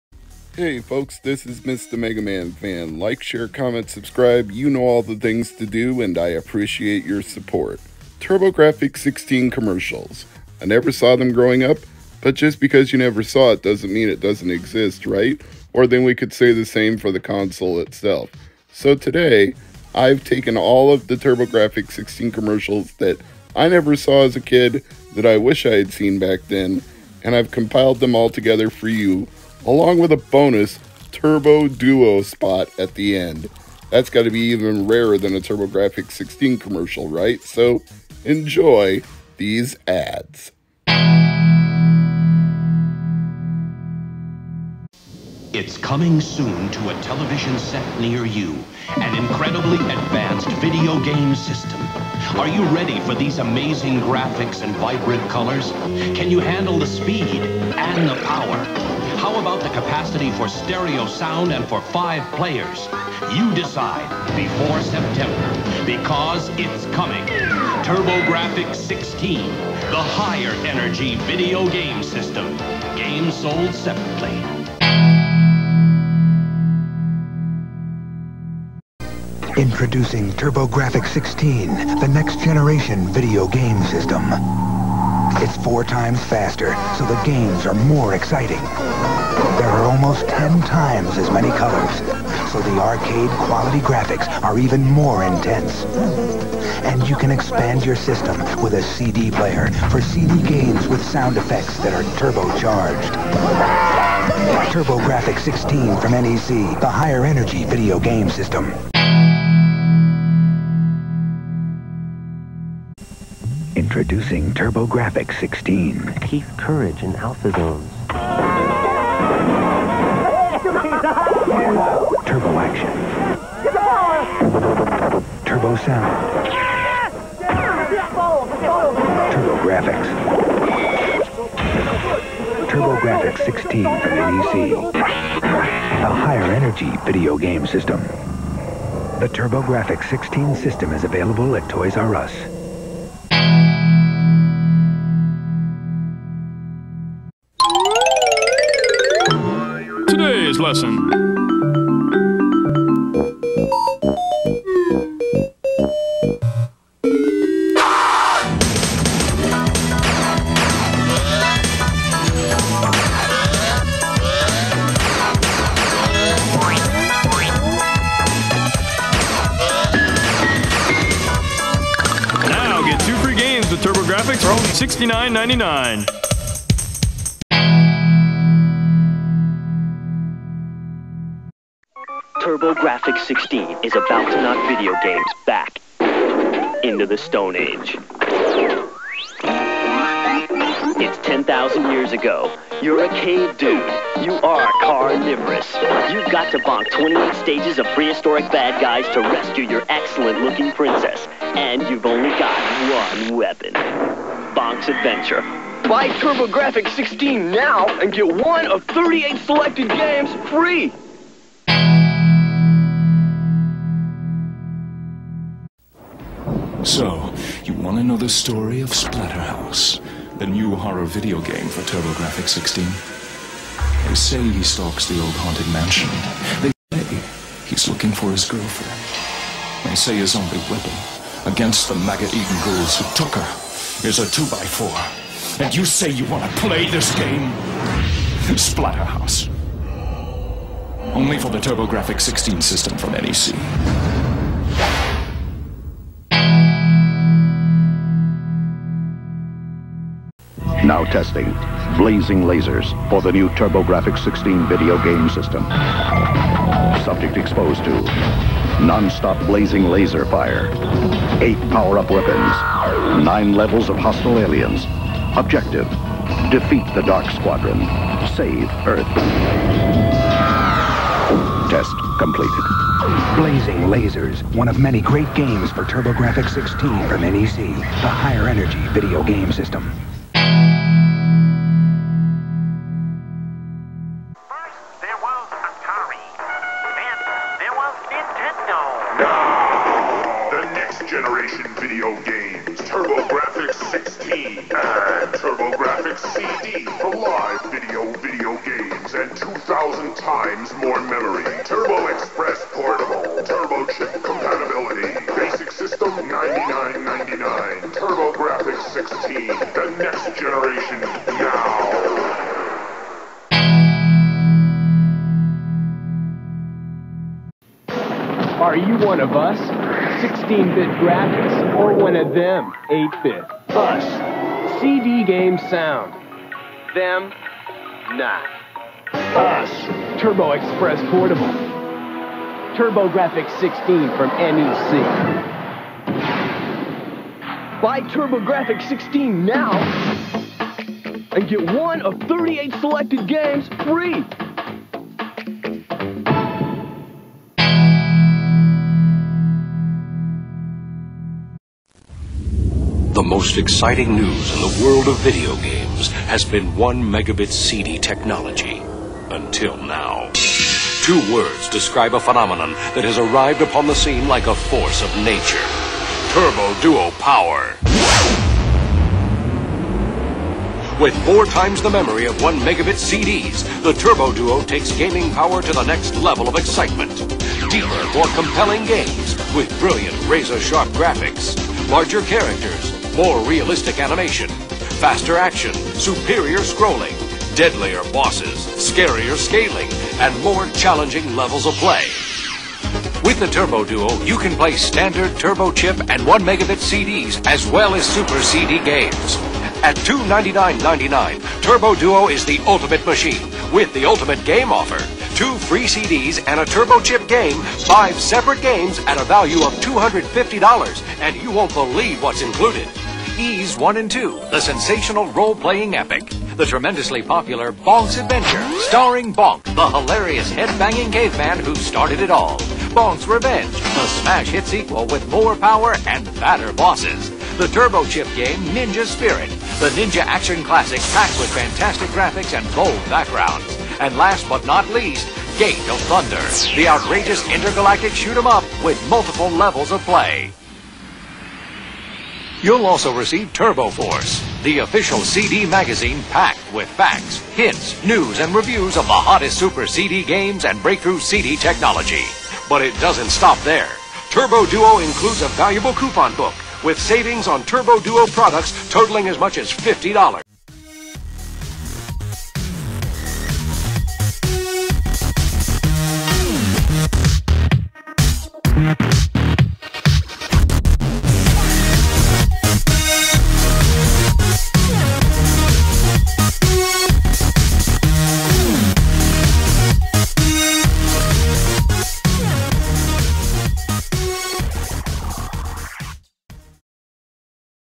TurboGrafx-16 Ads on The PC Engine Files
Where were all of these TurboGrafx-16 ads when I was growing up? I honestly can't recall ever having seen a single one of them on TV.